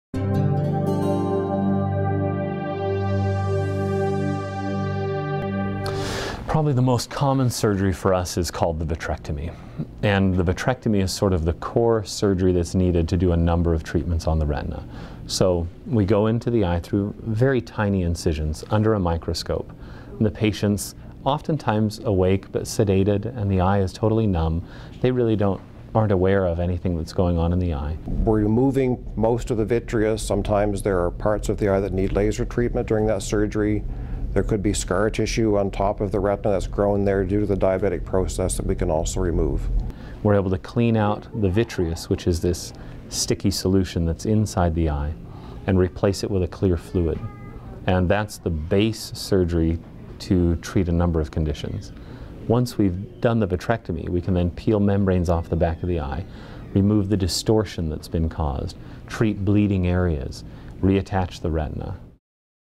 Arizona Retina & Vitrious Surgery Education - Barnet Dulaney Perkins Eye Center (AZ)
In this video, Dr. Jordan Graff & Dr. Brian Horsman discuss the general process for cleaning out vitrious solution from the retina and replacing it with a clear fluid.